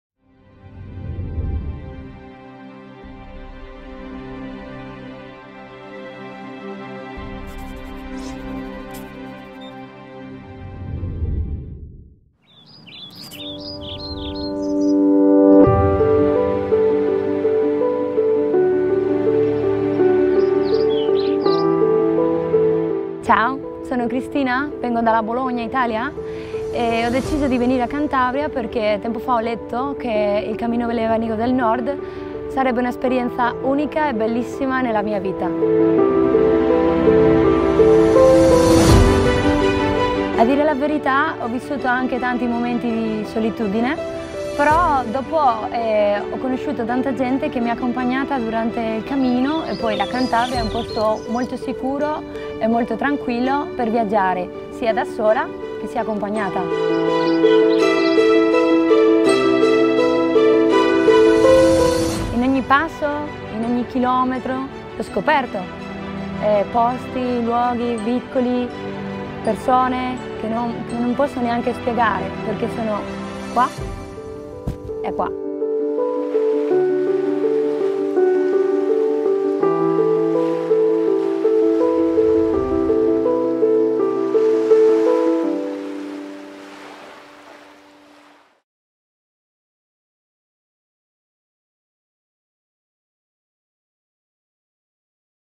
Tu Cantabria, Camino
Cristina ha venido desde Bolonia en Italia para conocer Cantabria. Para ella esta tierra es El Camino.
Ha venido a peregrinar y conocer el Camino del Norte y el Camino Lebaniego